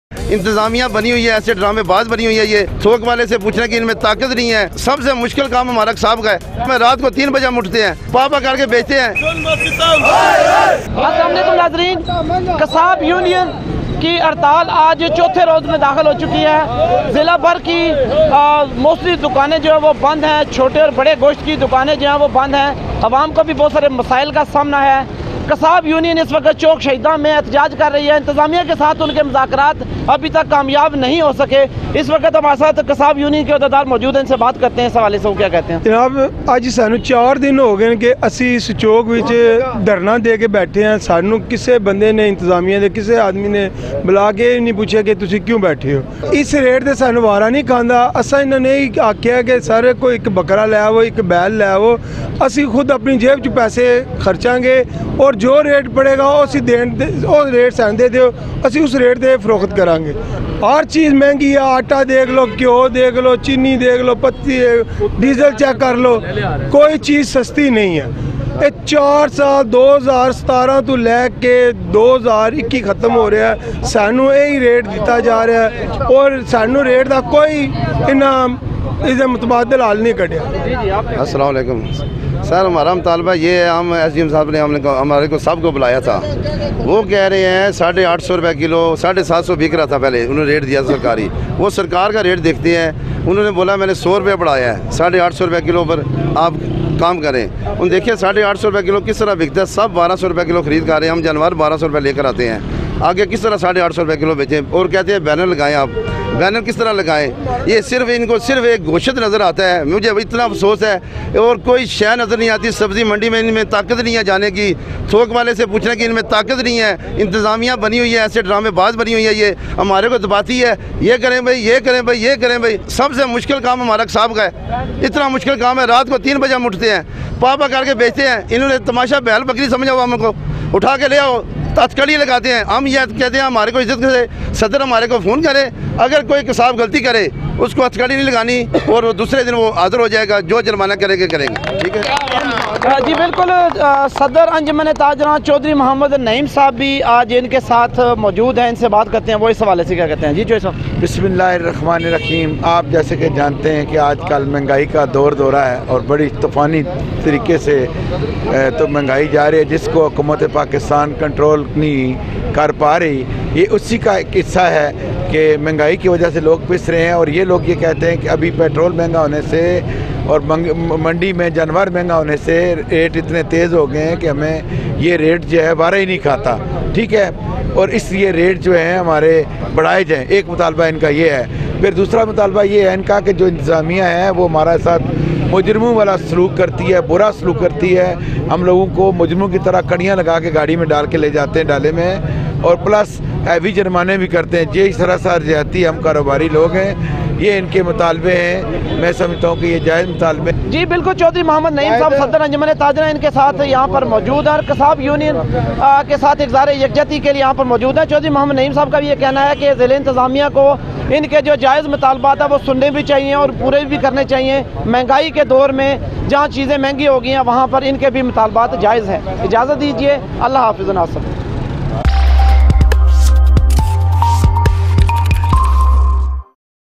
Kasab Union strike enters fourth day, full protest in Chowk Shaheed
قصاب یونین کی ہڑتال چوتھے روز میں داخل ،چوک شہیداں میں بھرپور احتجاج